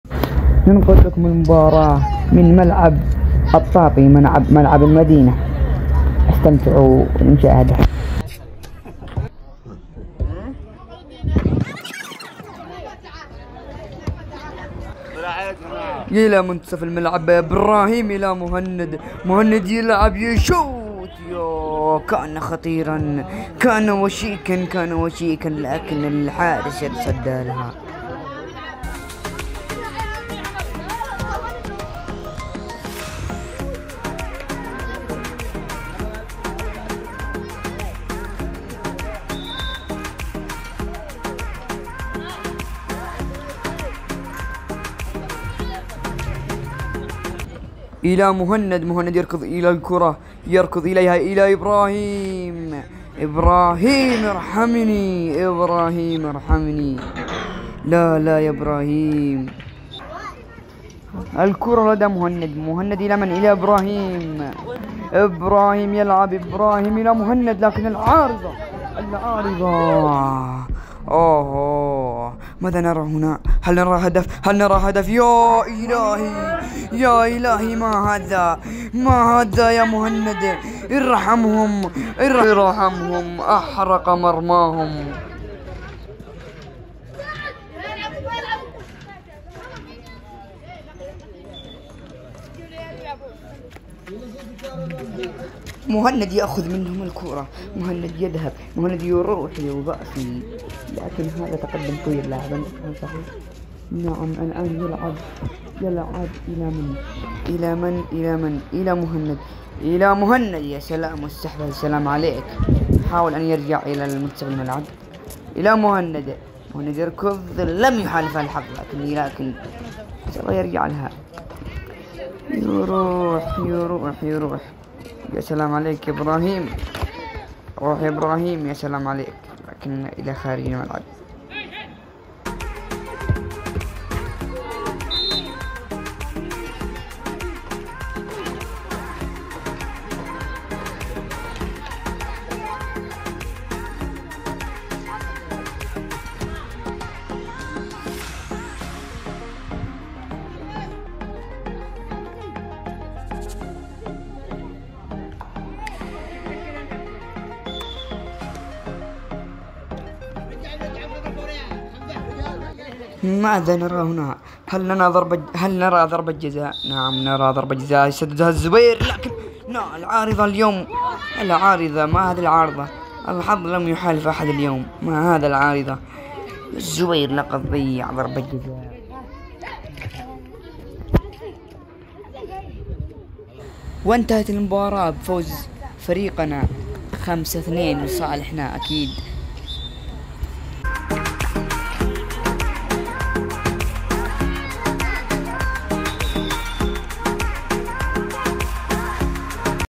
من ارض الحضاره مأرب | مباريات وتدريبات وديه
تدريبات ومباريات وديه بين الفرق من ملعب المدينه مأرب